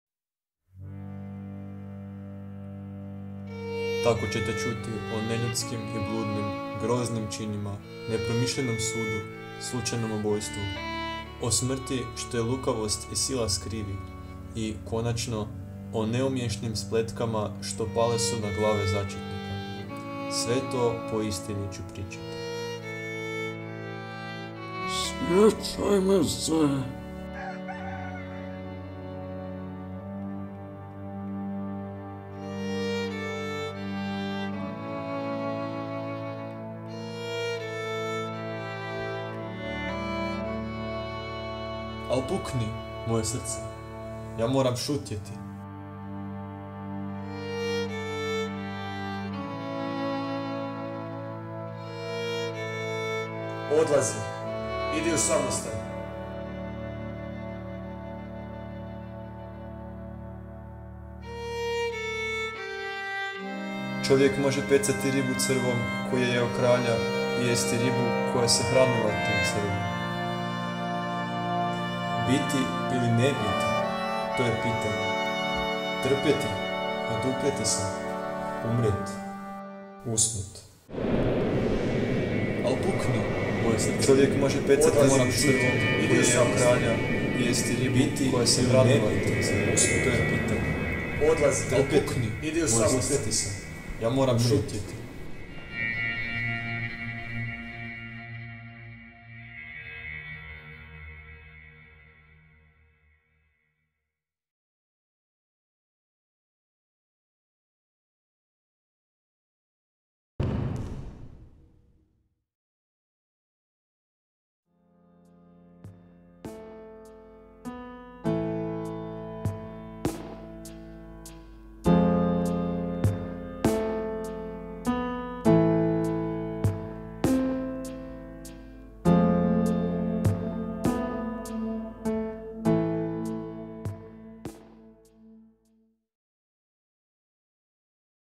Videonajava za djelo Hamlet (William Shakespeare), Gimnazija Josipa Slavenskog Čakovec
Videonajava je izrađena za CARNET-ovo natjecanje Kamera, ton, e-Lektira! u suradnji učenika/ca i nastavnika/ca Gimnazije Josipa Slavenskog Čakovec. Više o natjecanju pročitajte ovdje